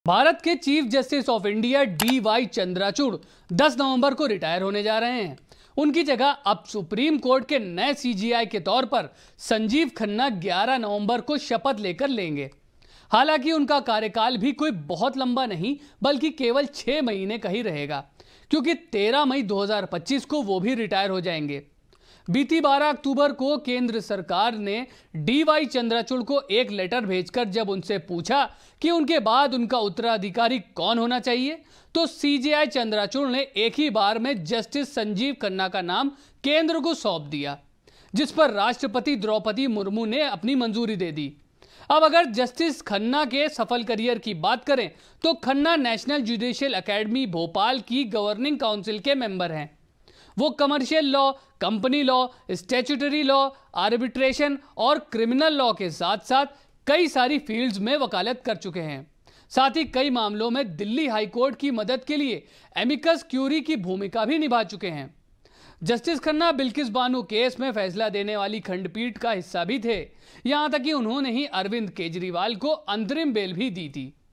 Justice Sanjiv Khanna बनेंगे नए Chief Justice of India
भारत के Chief Justice of India DY Chandrachud 10 नवंबर को retire होने जा रहे हैं। उनकी जगह अब सुप्रीम कोर्ट के नए CJI के तौर पर संजीव खन्ना 11 नवंबर को शपथ लेकर लेंगे। हालांकि उनका कार्यकाल भी कोई बहुत लंबा नहीं बल्कि केवल 6 महीने का ही रहेगा. क्योंकि 13 मई 2025 को वह भी retire हो जाएंगे.

DEN- 157
SITI –85
JGD-99